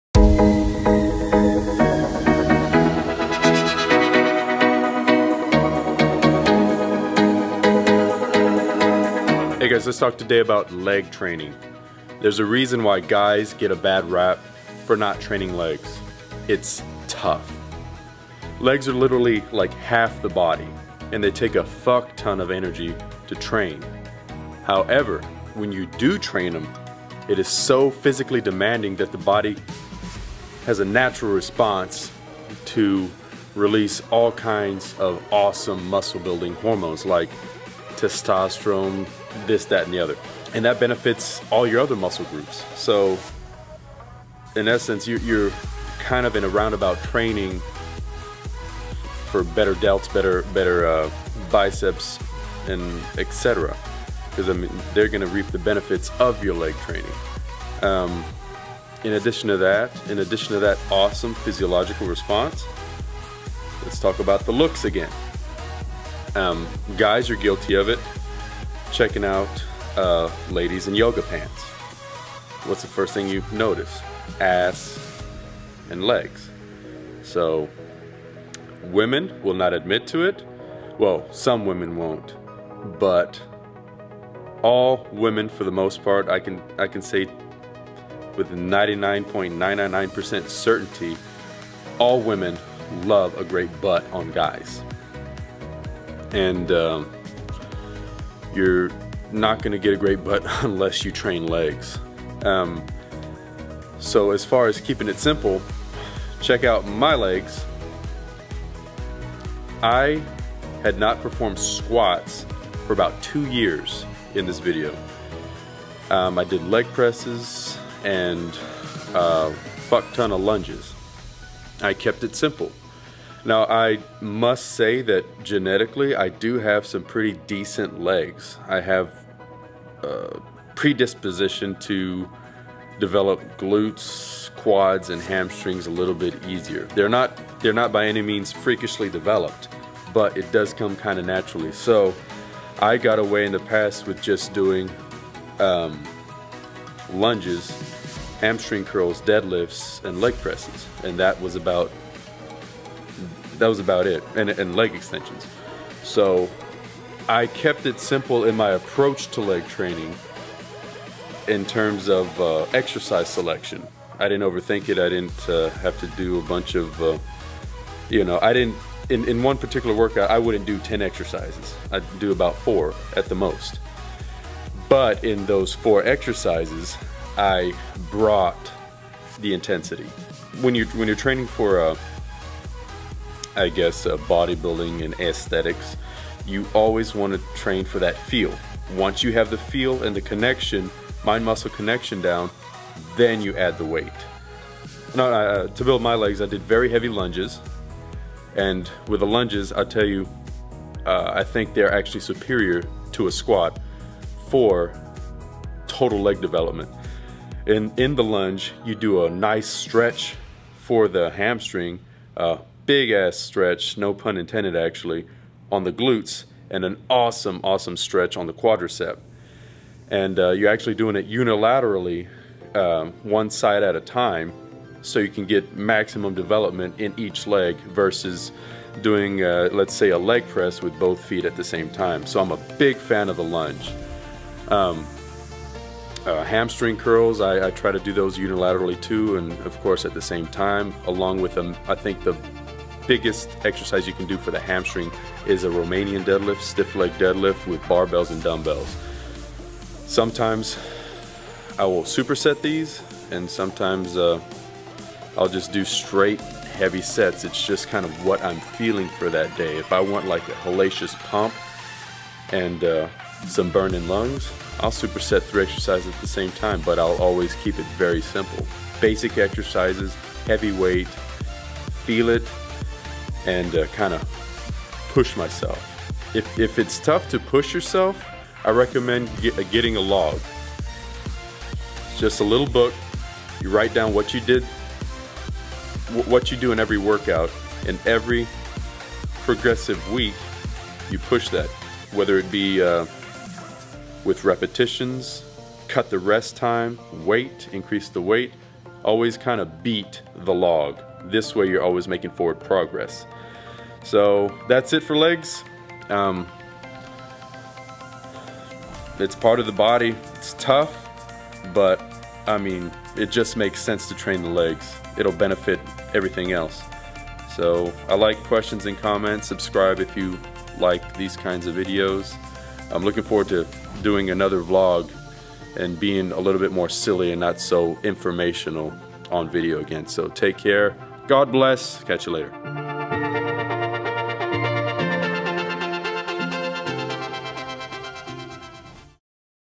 Quad Sweep Hamstrings and Glutes - Simple and Effective Leg Training
This is the condition of my legs after not squatting for 2 years. The squat just didn't agree with my knees and back. I did however go ape shit crazy on some lunges and leg presses and romanian deadlifts.
This is one the simplest leg workouts that I LOVE:
Superset for 7 sets:
Leg extensions 20 reps/walking dumbbell lunges 24 steps and deadlifts 12-15 reps.
Rest for 2-3 minutes after the deadlifts then repeat 7 times!